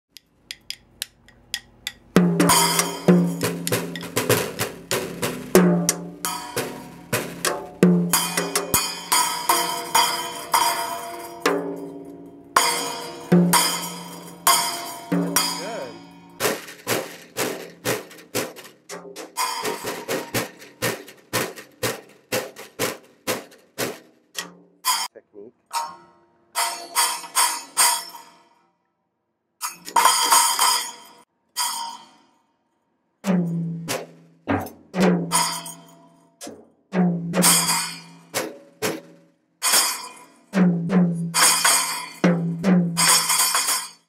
Shaw Drumming at 3 2/3rds years old
No lessons yet. First drum set Xmas day 2015.  I've given him a very few tips and he's watched a couple of in-person drummers, and daily he practices with chop sticks on a counter while listening to the Rolling Stones. achydad.com